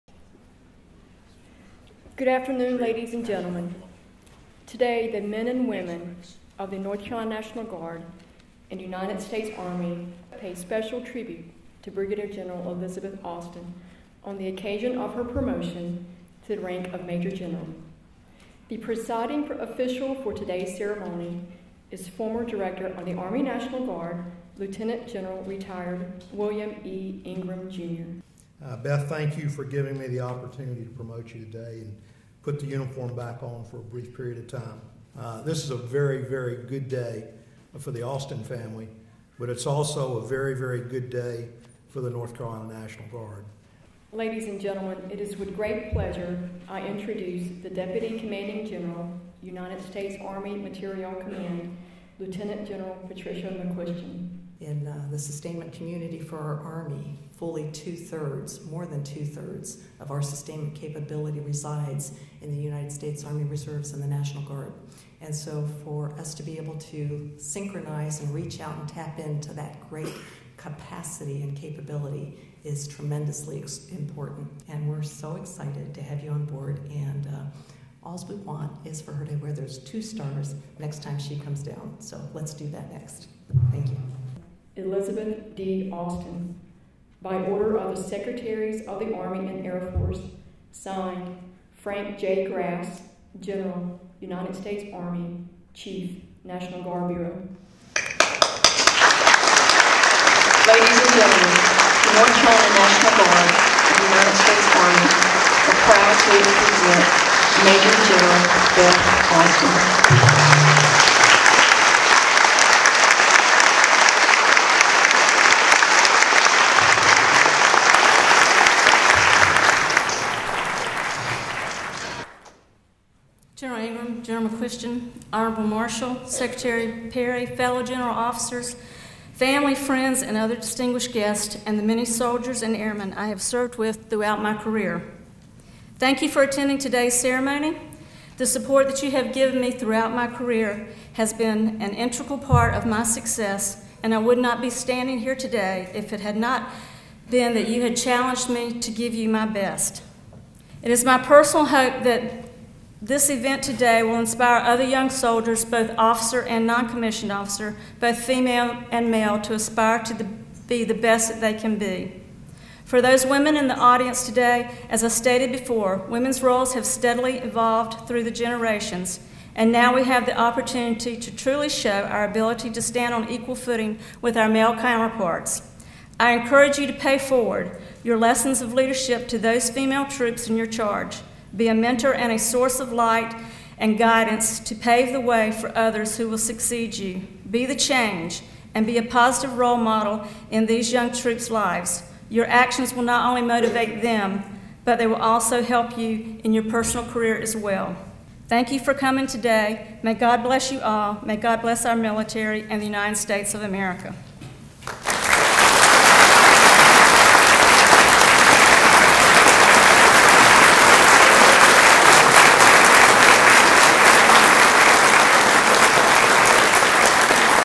Maj. Gen. Austin Highlights
Major General Elizabeth Austin receives her second star at a promotion ceremony held at Joint Force Headquarters in Raleigh North Carolina. She is the first female Major General to be promoted from the ranks of the North Carolina National Guard. Retired Lt. Gen. William E. Ingram Jr. and Lt. Gen. Patricia E. McQuistion speak as distinguished guests.